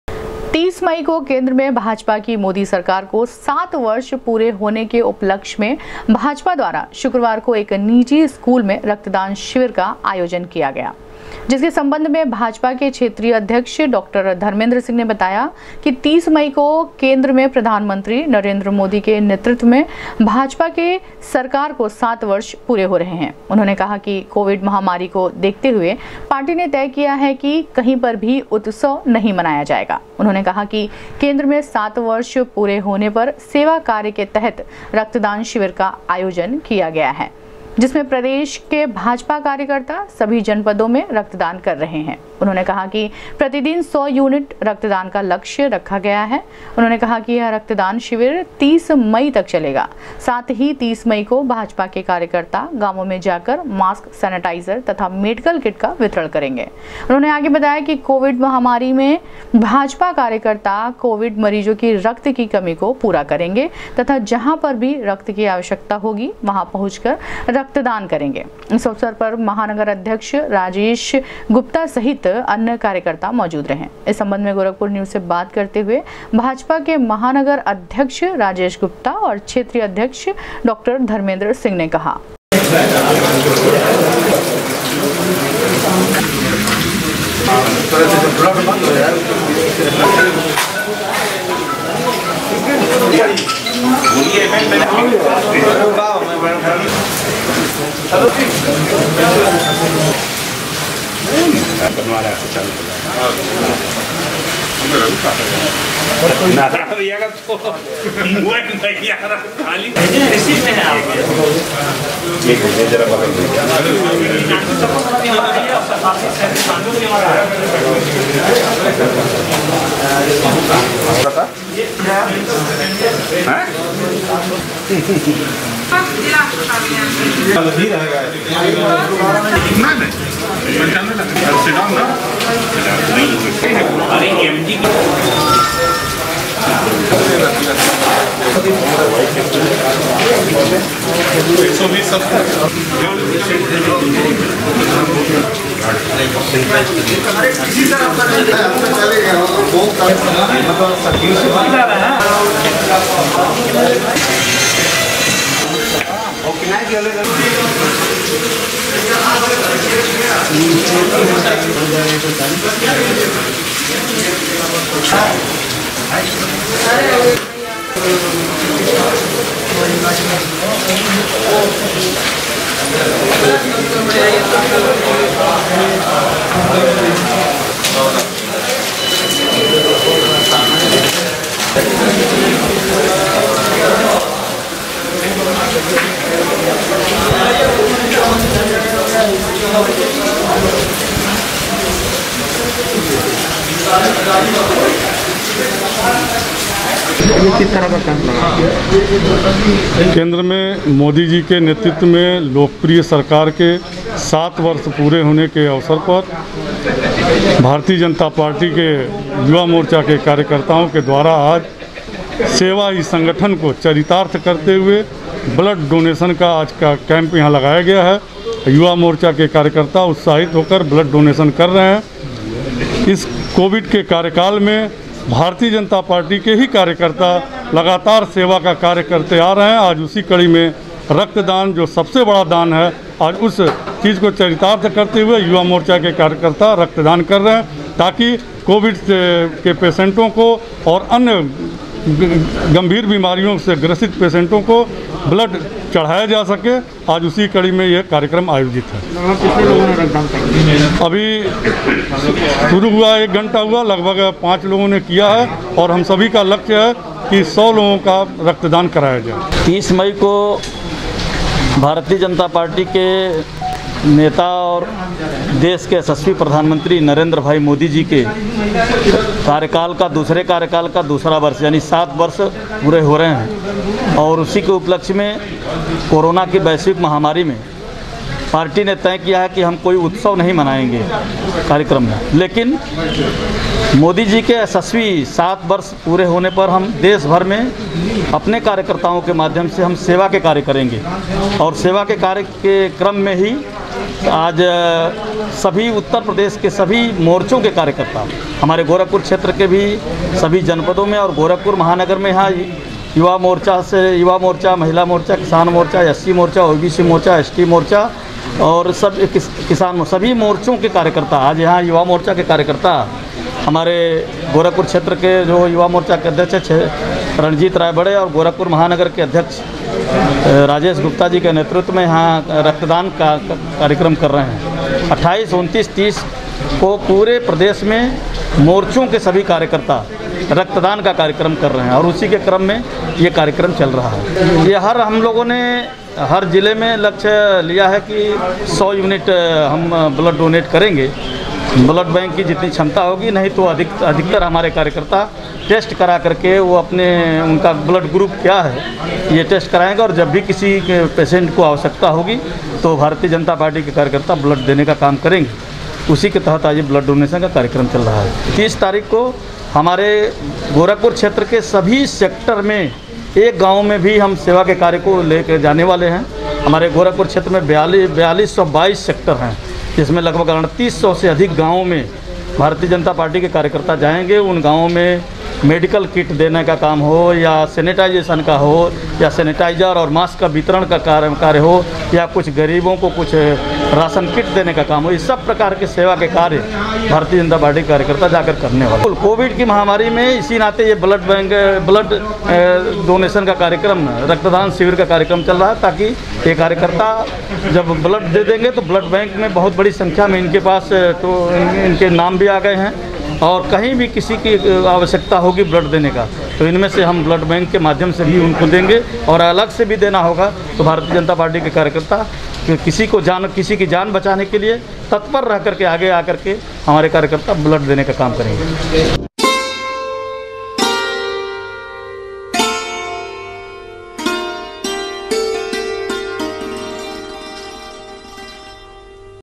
30 मई को मोदी सरकार के सात वर्ष पूरे होने पर भाजपाईयो ने किया तीन दिवसीय रक्तदान शिविर का आयोजन
30 मई को मोदी सरकार के सात वर्ष पूरे होने पर भाजपाईयो ने किया तीन दिवसीय रक्तदान शिविर का  आयोजन,कार्यकताओं ने बढ़ चढ़कर किया रक्तदान,क्षेत्रीय अध्यक्ष ने कहा,कोविड मरीजों के इलाज में सहायक होगा रक्तदान